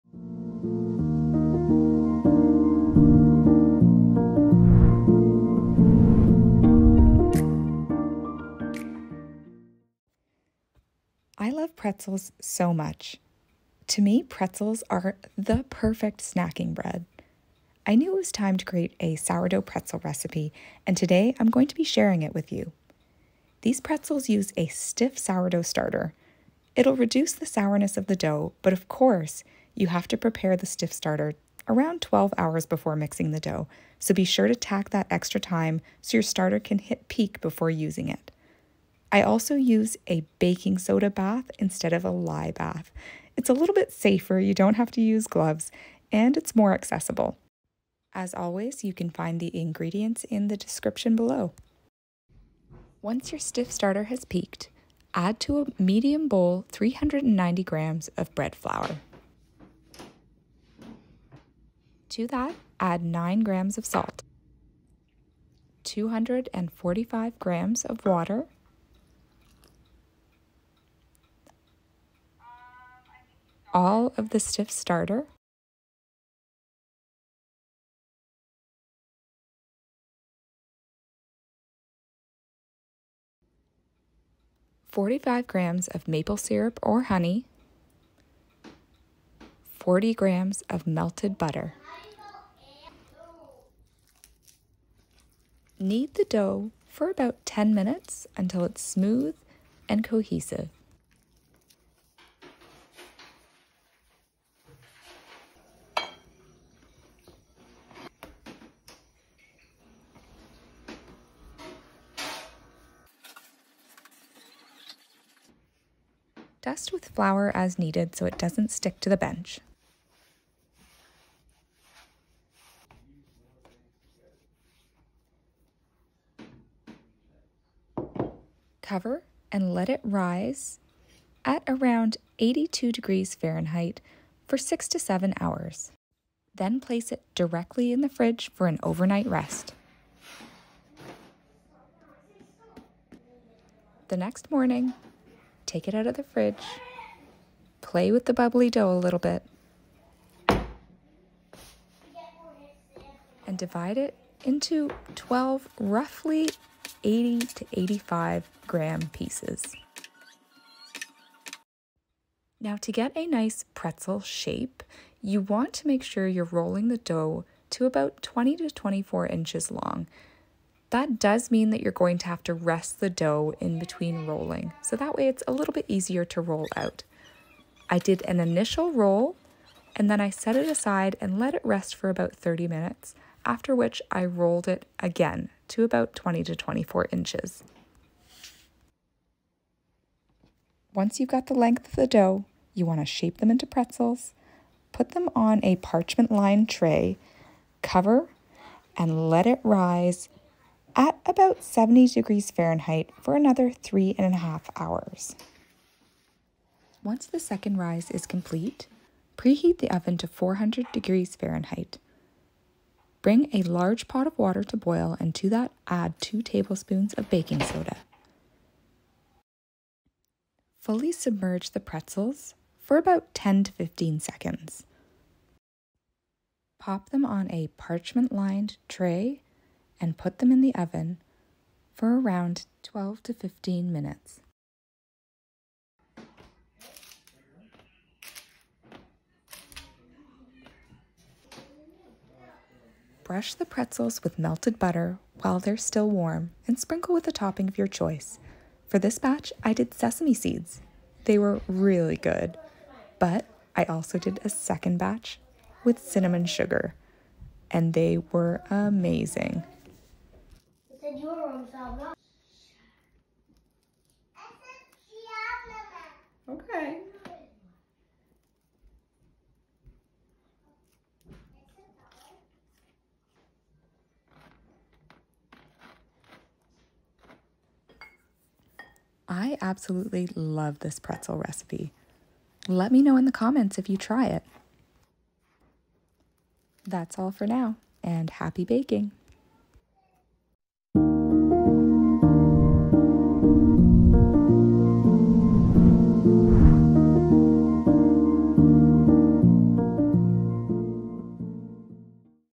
The only sourdough pretzel recipe you need! Homemade sourdough pretzels!
This soft pretzel recipe is a smash!  It will make 12 standard soft pretzels. Let me know what you think of it in the comments!

Be sure to prepare the sweet stiff starter about 12hrs BEFORE starting the dough.  Mix all of the starter ingredients until fully combined.  Cover and let it rise until peak, and mix the dough when the starter has reached peak.

Sweet stiff starter
50g 100% hydration unfed sourdough starter
66g water
120g flour
20g sugar

Dough
390g bread flour
245g water
45g honey or maple syrup
40g butter
9g salt
All stiff starter

Oven is preheated to 400F, and pretzels baked 12-15 mins!